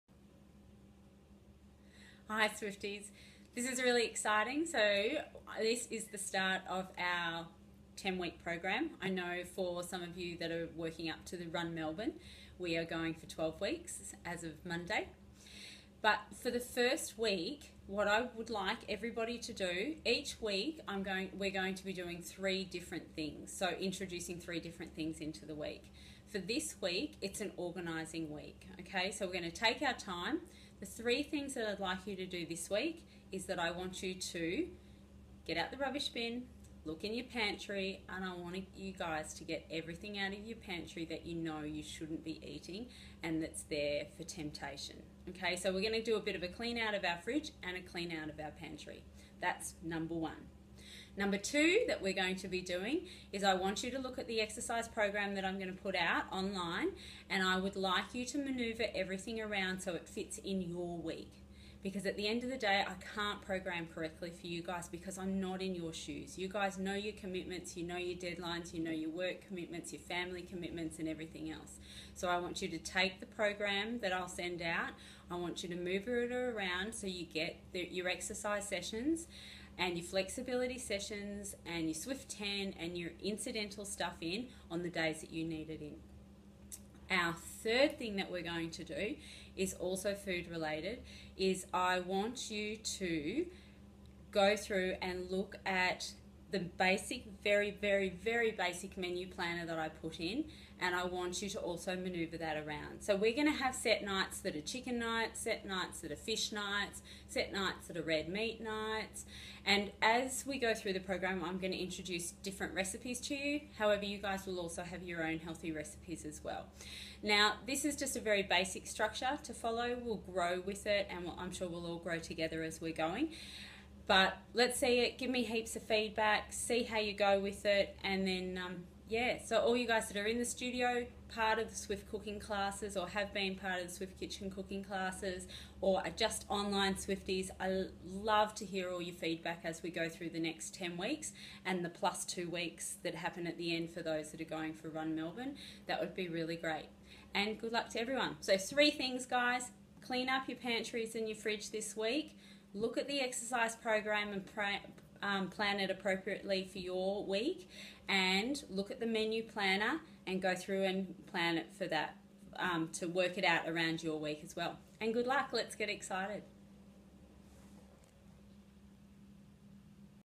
Swift 10 week program - week 1 intro
Swift 10 week program is a healthy lifestyle program for people who are busy and need flexibility in there health and wellness journey. We introduce 3 swift healthy actions each week. In week 1 we will be sorting out the pantry and fridge to get out those "temptations", we will be planning our weekly exercise/movement and meal routine - let's get excited because right now is the best time to just "Get on with it and make it count"